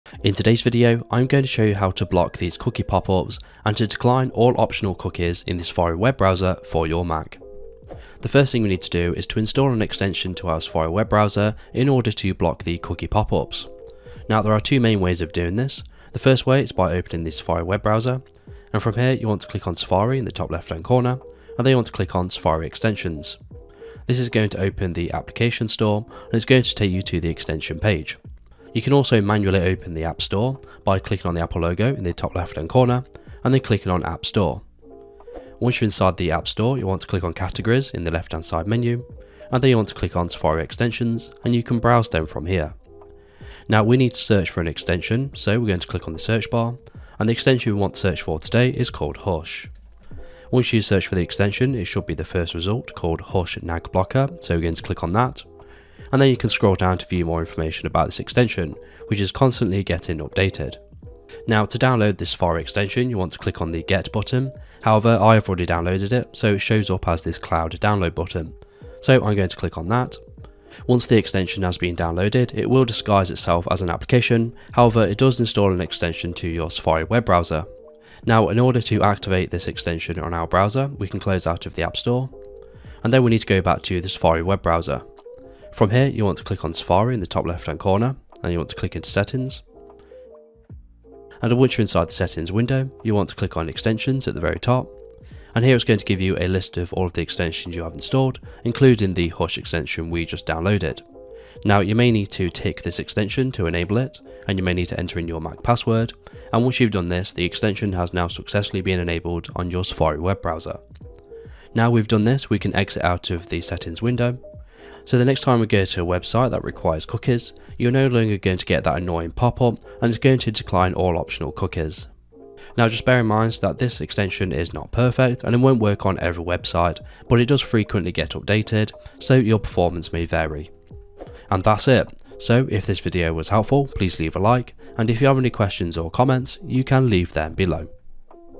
How To Block Cookie Pop Ups in Safari On Mac | Easy Guide
Today, you will successfully learn how to block most cookie pop-ups and automatically deniy optional cookies in Safari on Mac. Blocking cookie pop-ups streamlines your browsing experience by eliminating annoying interruptions on websites. It enhances privacy by avoiding unnecessary consent and prevents accidental acceptance of invasive tracking cookies that collect your data for targeted ads or third-party analytics.